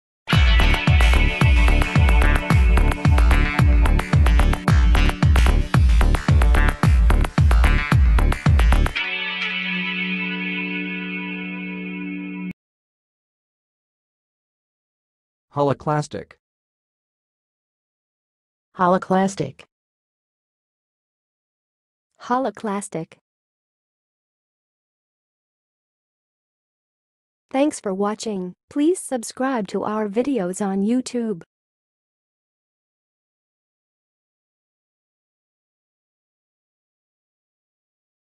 How to say "holoclastic"! (High Quality Voices)
Watch in this video how to say and pronounce "holoclastic"!